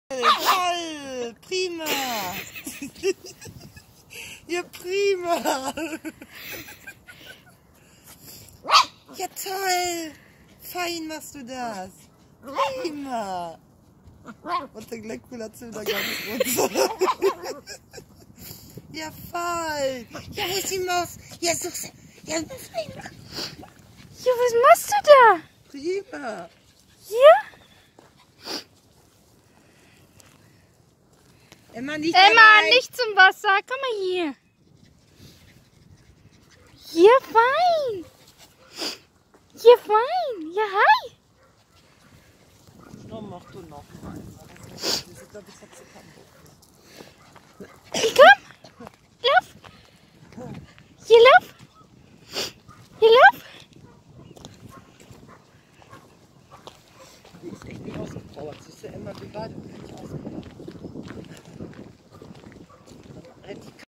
Die Hündin Emma dreht durch beim spazieren 😱🤣🤣🤣
Als ich (Pilus) mit einer Freundin und Emma spazieren war, am Rhein-Herne Kanal drehte die Hündin Emma plötzlich vollkommen durch und fing an sich auf einer schräge an zu wälzen wo ganz viel Moos drauf lag. 😱🤣🤣🤣🤣🤣 Ab und an rollte sie dann auch ein kleines Stückchen herunter was sehr witzig aussah.
Vielleicht erfreut du dich ja genauso an diesem kleinen Video wie wir es getan haben. Deine Pilus 👧🏻❤